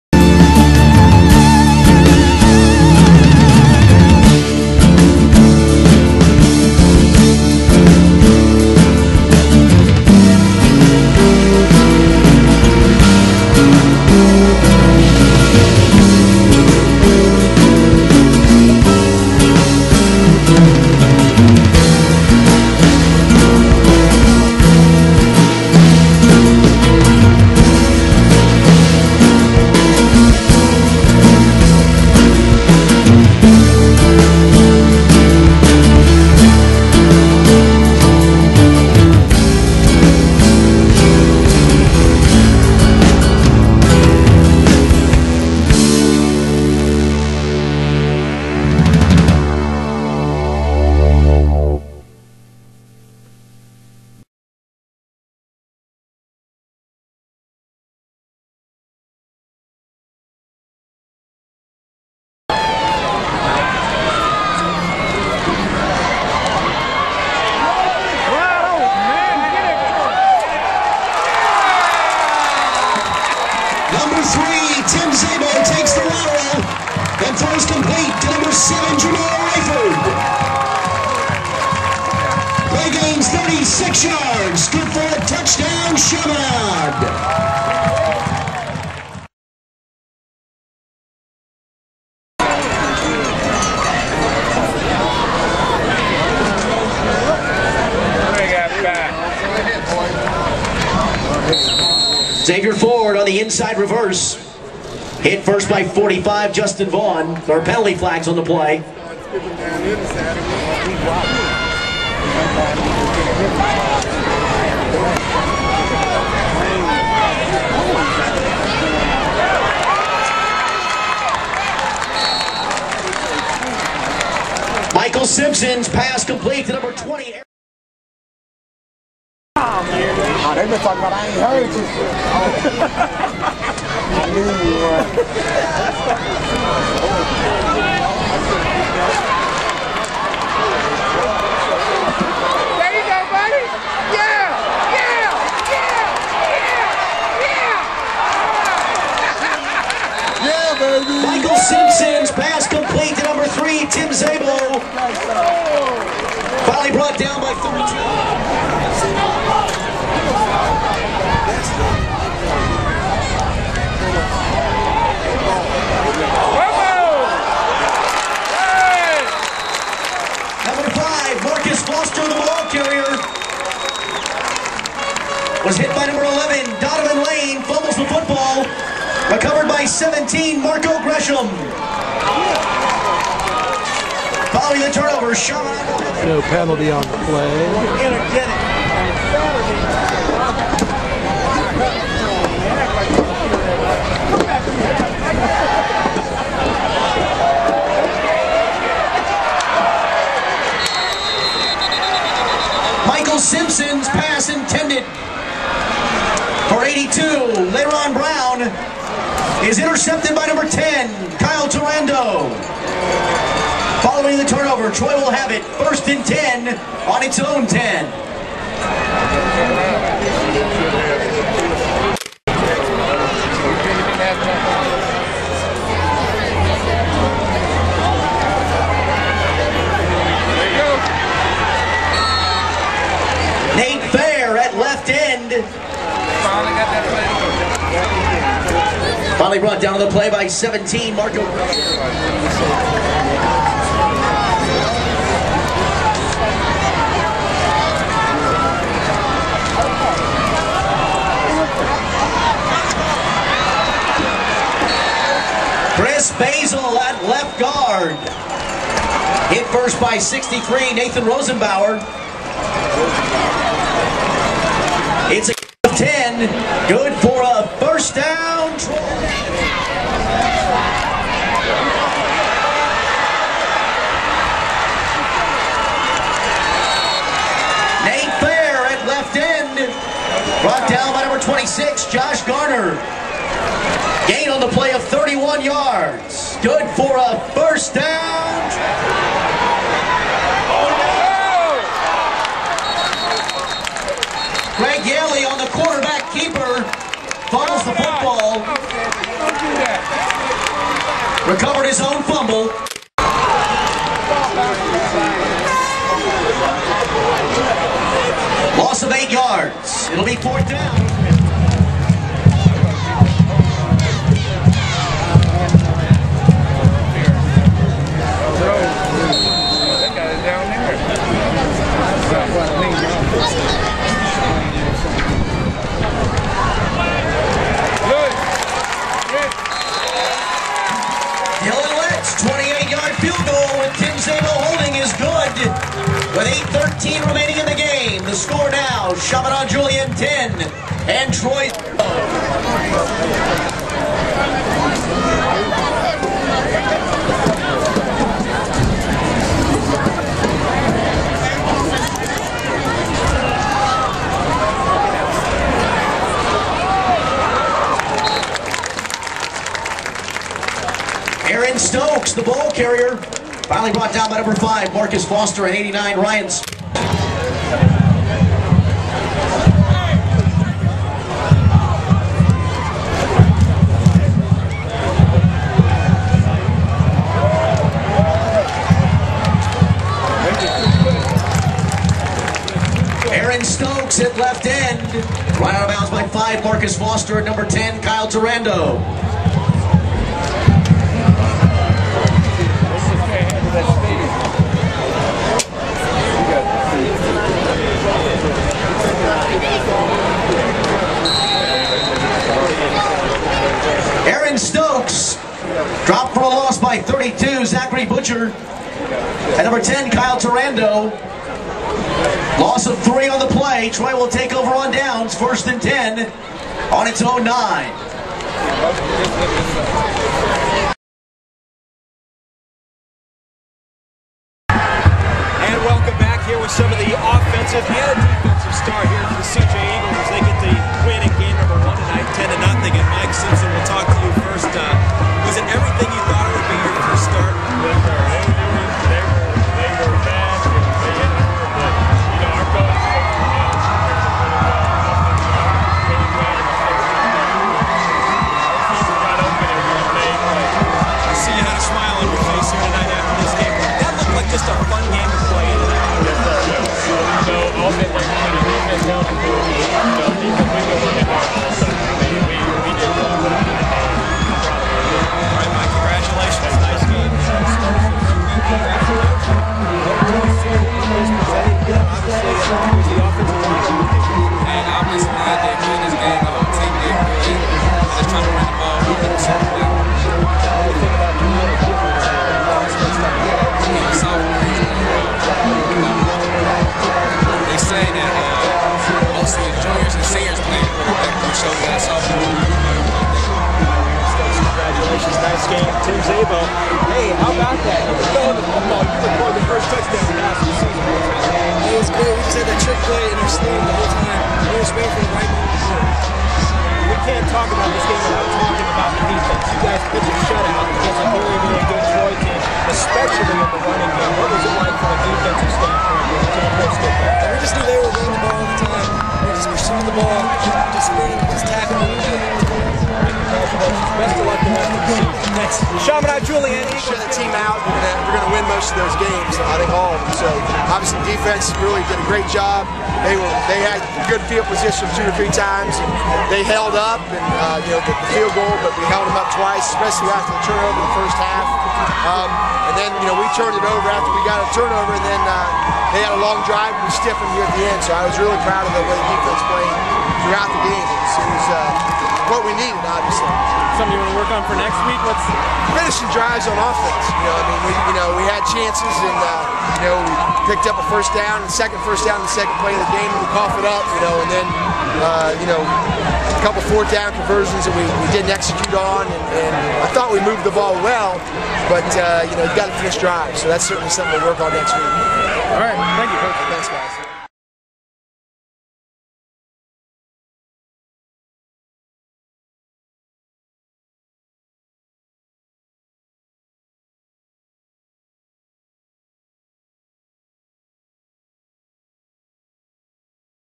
MVPpostgame--EXTRA--Troy v. Chaminade Julienne
Watch clips from the first high school football game played in the area for 2009-Troy v. Chaminade Julienne at the Skyline Chili Crosstown Shootout, Welcome Stadium on August 27. The final score? CJ 10, Troy 0.